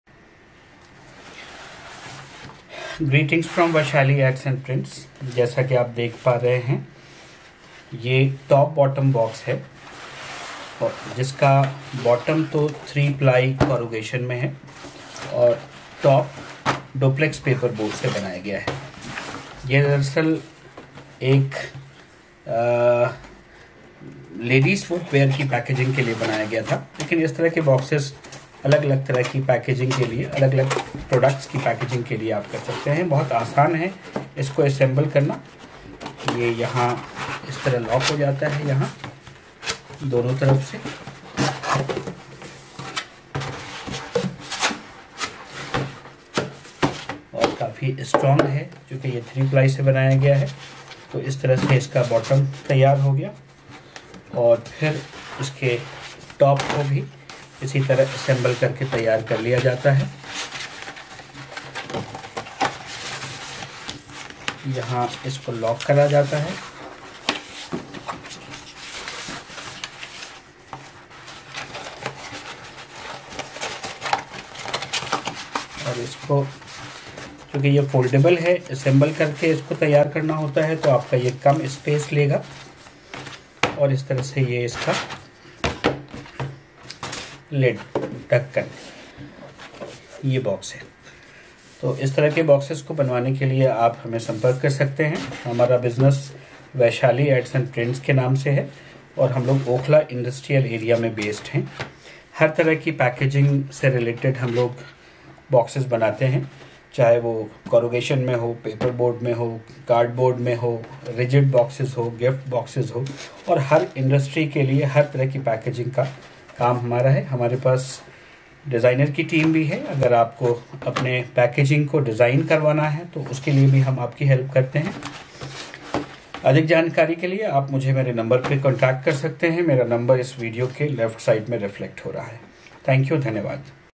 Footwear Packaging Box
Top : Printed on Duplex Paper with Matte Lamination
Bottom: 3 ply Corrugated

Made By
Vaishali Ads & Prints
140, DSIDC Shed, Okhla Industrial Area, Phase-1, New Delhi- 110020

GSTIN: 07AIWPA1628G1ZA